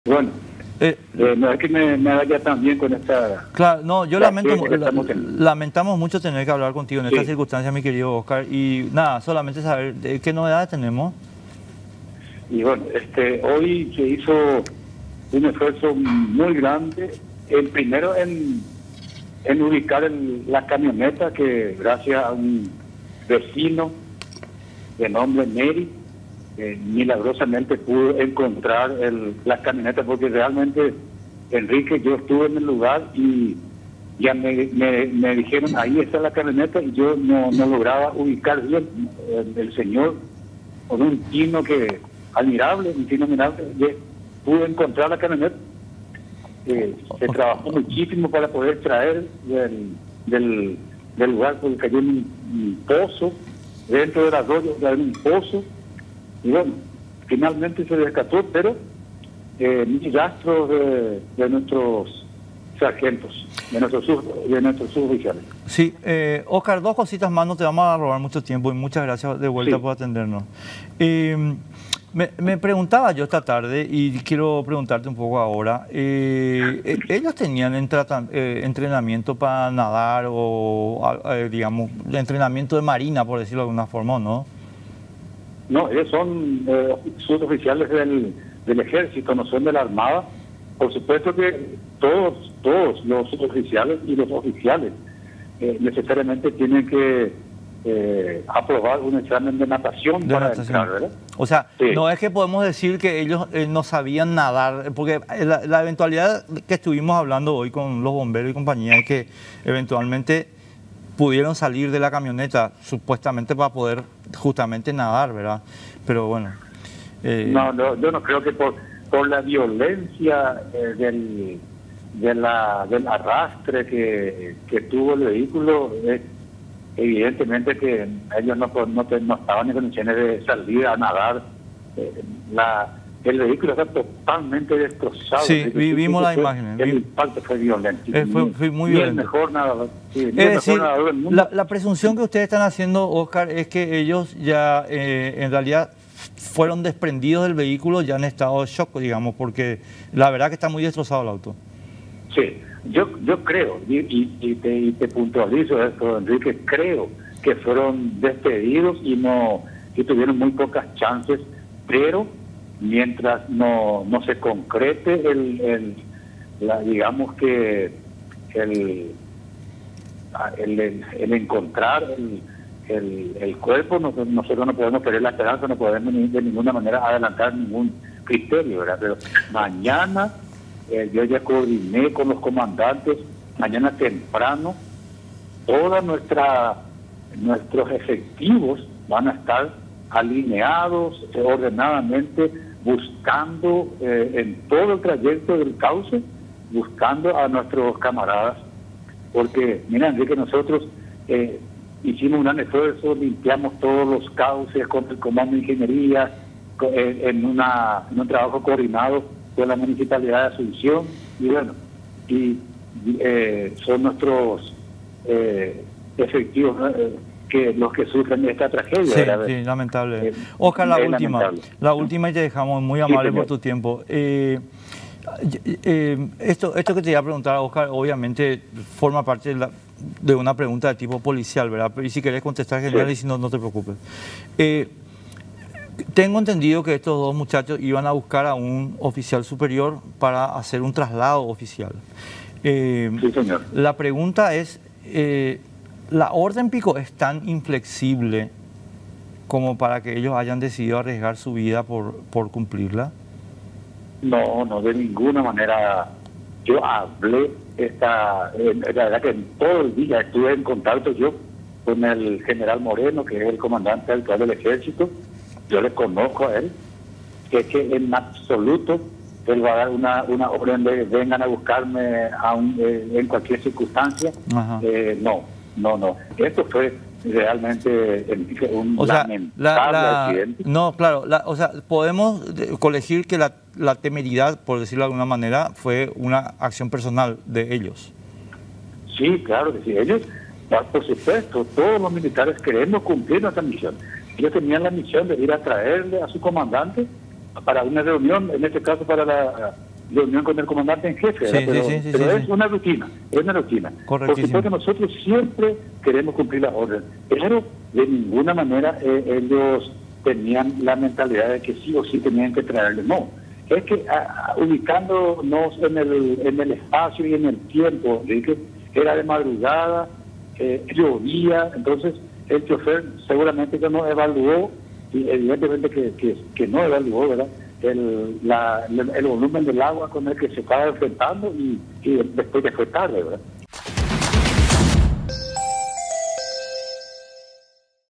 Militares arrastrados por el raudal: sigue la búsqueda de los desaparecidos
"Por la violencia del arrastre que tuvo el vehículo, evidentemente ellos no tuvieron las condiciones para salir a nadar, el vehículo está completamente destrozado", expresó el ministro de Defensa, Óscar González. Equipos de la Armada Paraguaya continuarán mañana viernes con la búsqueda de los dos militares desaparecidos esta madrugada tras el raudal que los arrastró junto con la camioneta del Comando del Ejército en la que se desplazaban.